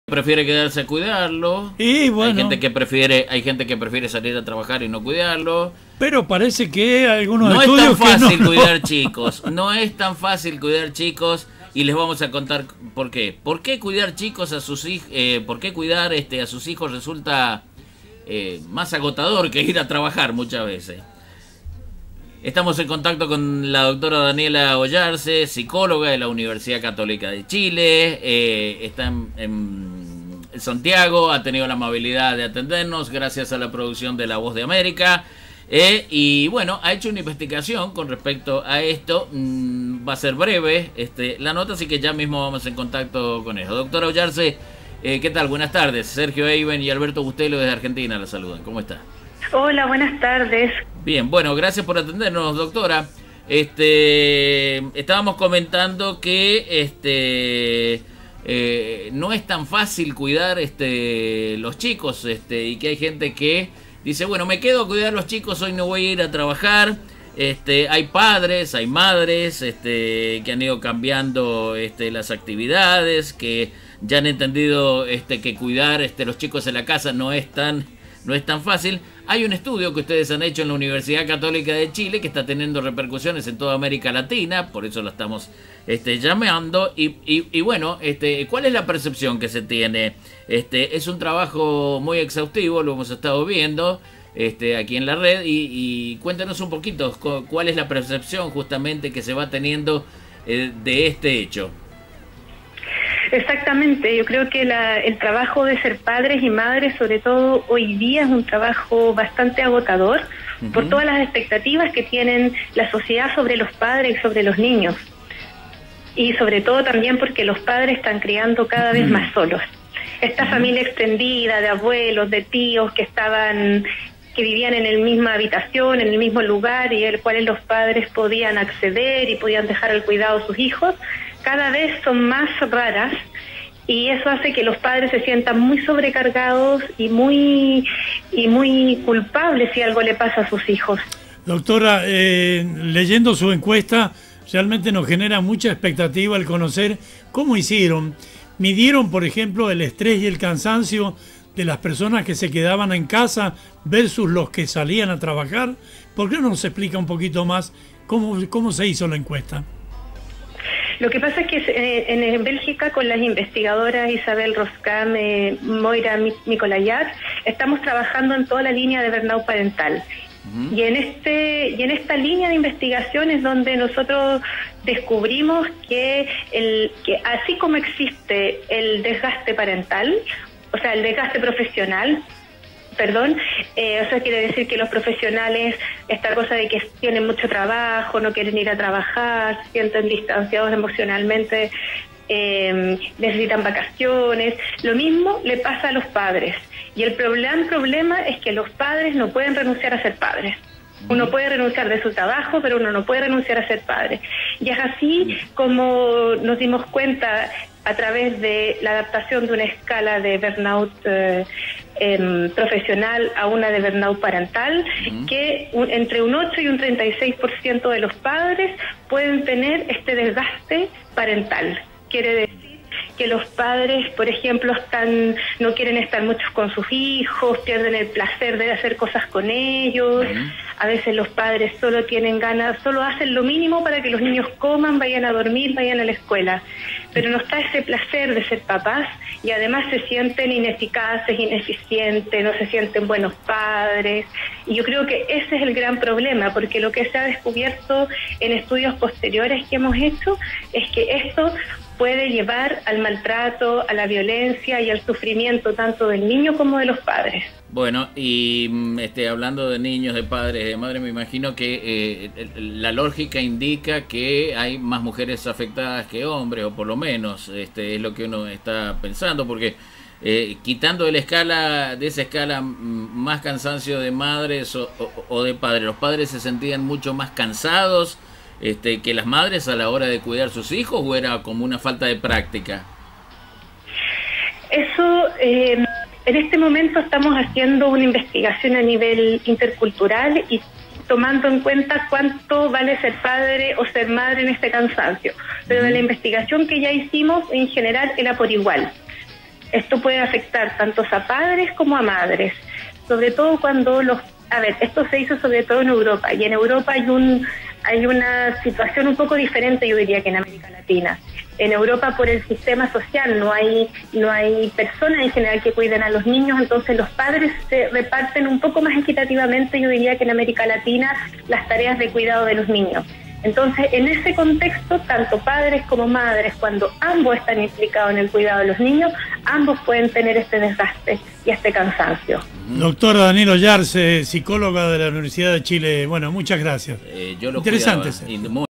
DRA. DANIELA OYARCE
¿POR QUÉ CUIDAR A SUS HIJOS RESULTA MÁS AGOTADOR QUE IR A TRABAJAR?